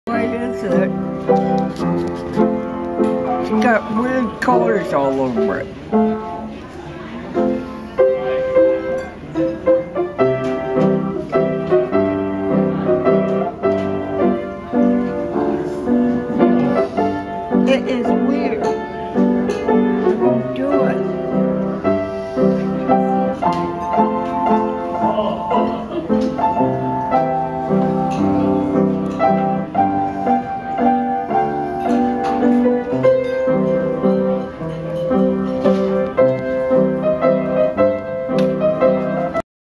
Hazel takes a picture.
Hazel bought a new camera last week at Radio Shack and was trying it out at the senior center this morning.